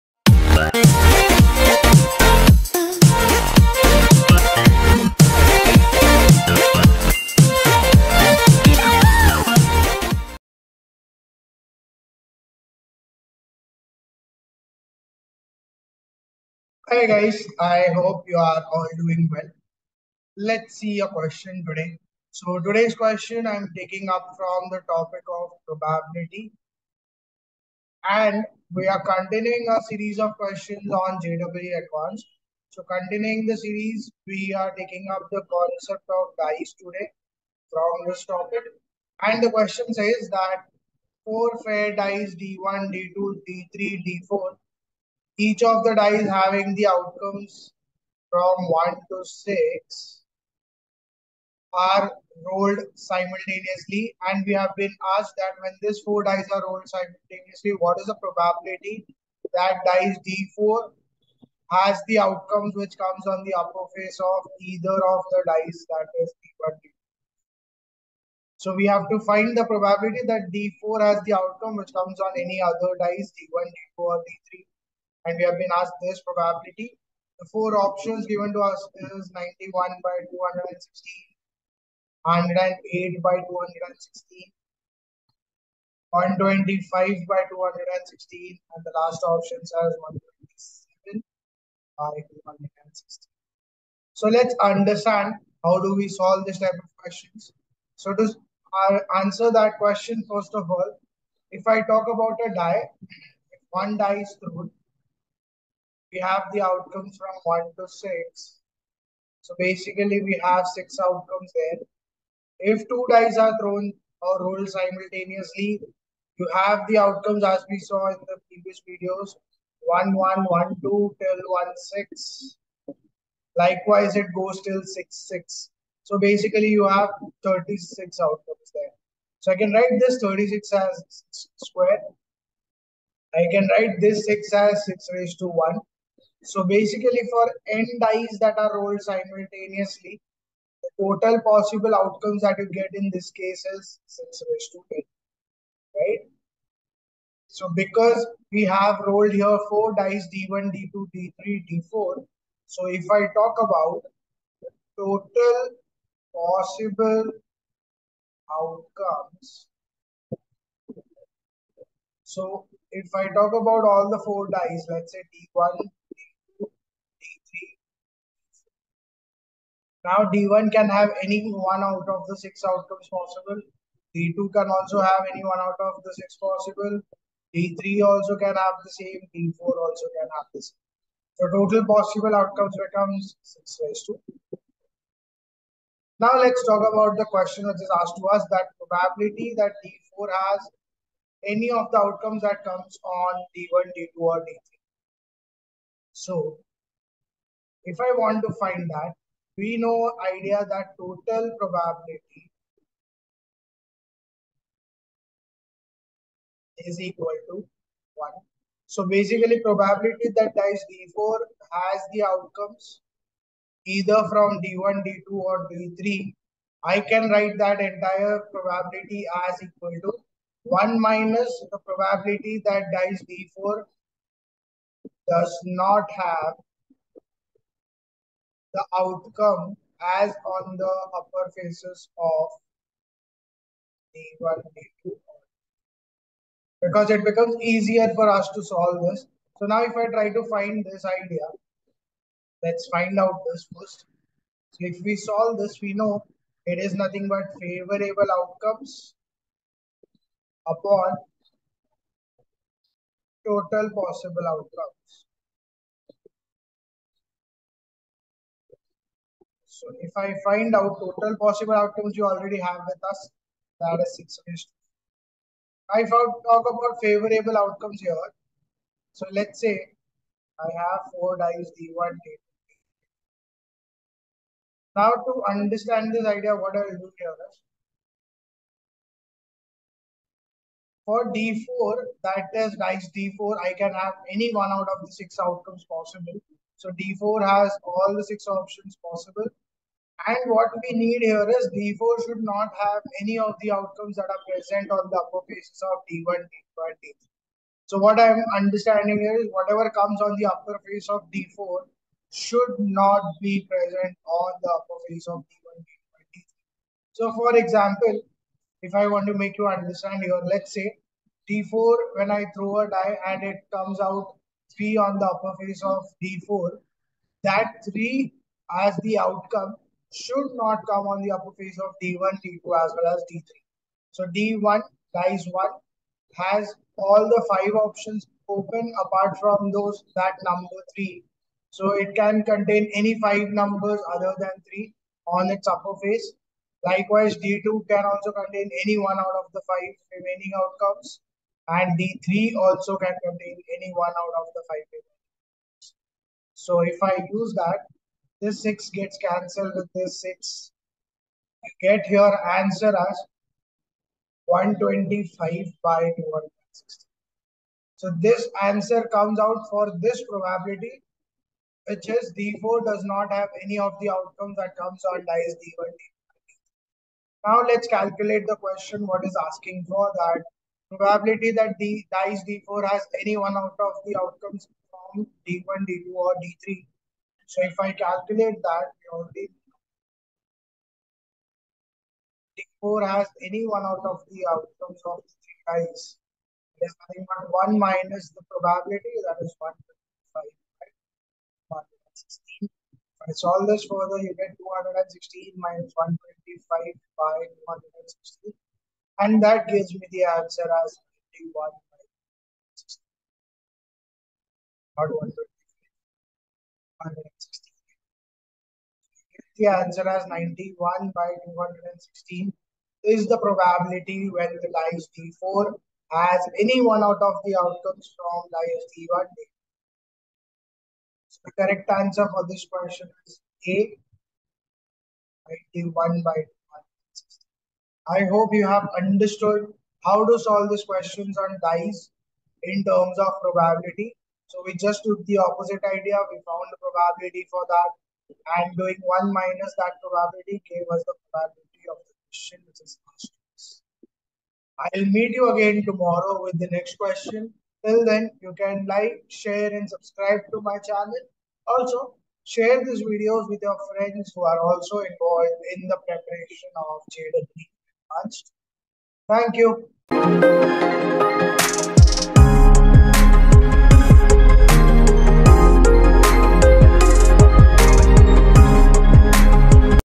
Four fair dice D1,D2,D3 and D4 each having 6 faces numbered from 1,2,3,4,5 and 6 are rolled | JEE
Four fair dice D1, D2, D3 and D4 each having 6 faces numbered from 1,2,3,4,5 and 6 are rolled simultaneously. The probability that D4 shows a number appearing on one of dice D1, D2 and D3 is: ?
A. 91/216
B. 108/216
C. 125/216
D. 127/216